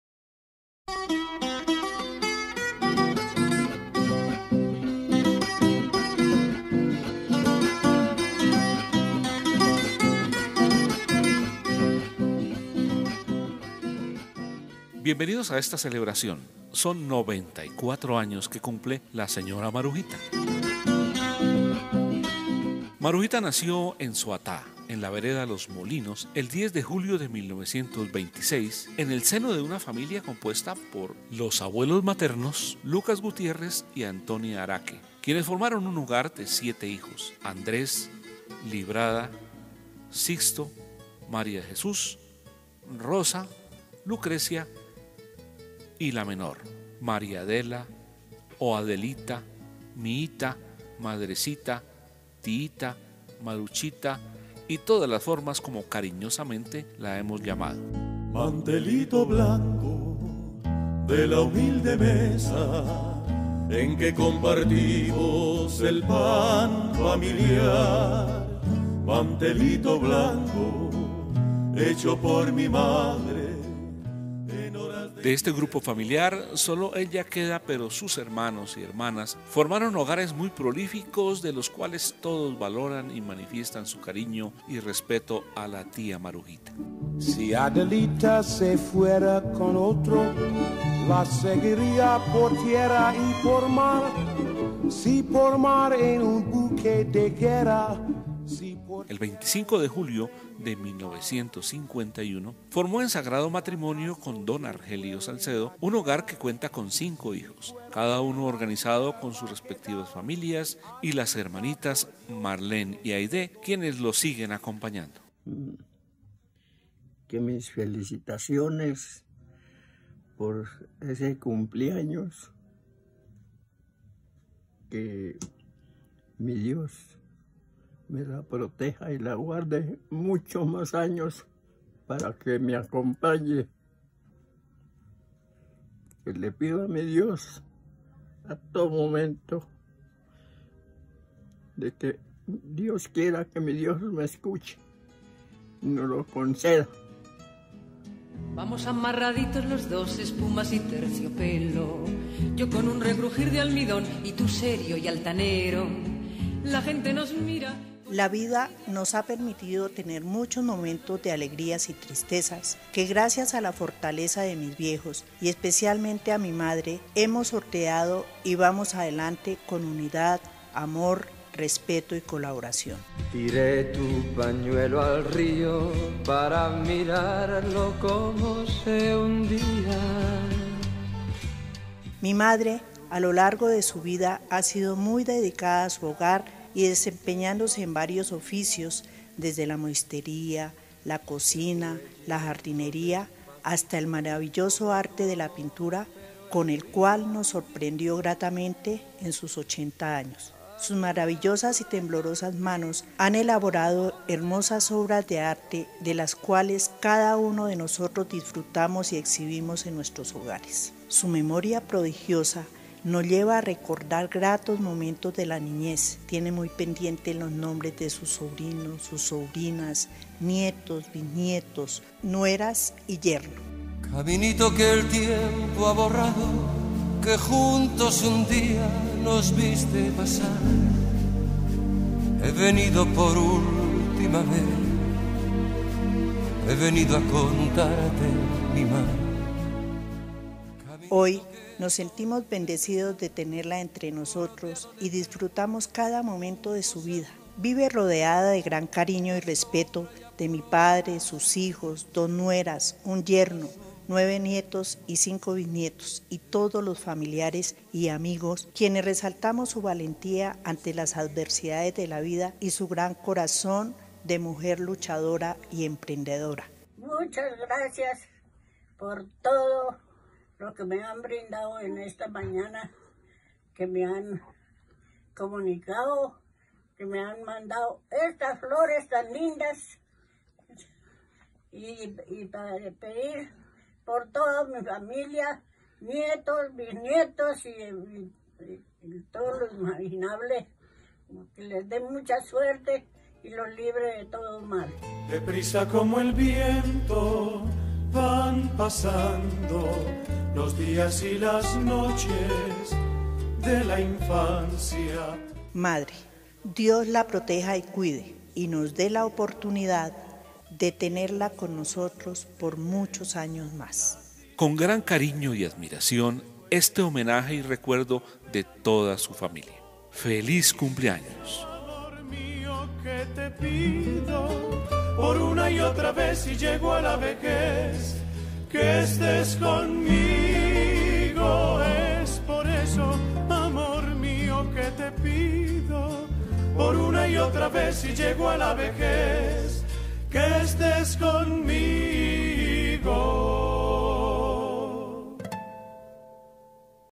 CUMPLEAÑOS MARUJITA 2020 OK
Este video es un homenaje a Marujita o Miita como cariñosamente la llamamos. 94 años acompañándonos con el ejemplo y fortaleza que le caracteriza. Dios la bendiga suegrita.